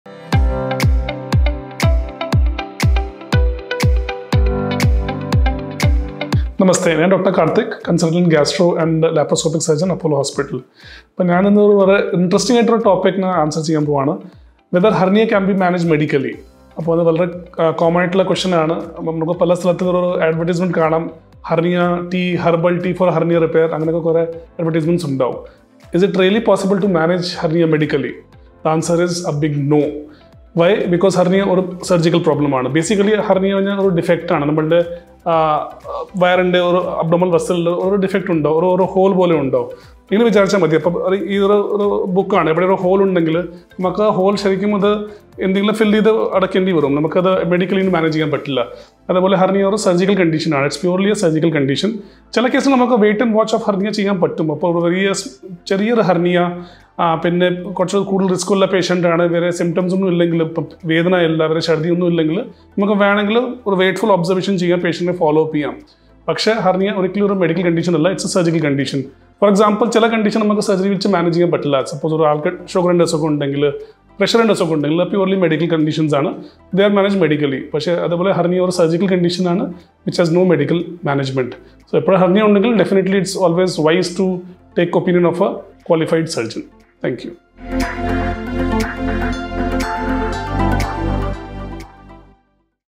Apollo Adlux Dr Kartik Myth Busted Video
നമുക്കിടയിൽ പലർക്കും ഉള്ള ഒരു തെറ്റിദ്ധാരണയാണ് ഹെർണിയ മരുന്നു കഴിച്ചു മാറ്റാമെന്ന്. ഹെർണിയ എന്താണെന്നു അറിയാത്തതുകൊണ്ടാണ് ഈ തെറ്റിദ്ധാരണ ഉളവെടുക്കുന്നത്. ഇന്നത്തെ മിത്ത് ബസ്റ്റഡ് സെഗ്മെന്റിൽ ഞങ്ങളുടെ സർജിക്കൽ ഗ്യാസ്ട്രോഎൻട്രോളജി വിഭാഗം കൺസള്റ്റന്റ് ഡോ. കാർത്തിക് കുൽശ്രേഷ്ഠ ഈ തെറ്റിദ്ധാരണ തിരുത്തി കുറിക്കുന്നു. വീഡിയോ കാണാം.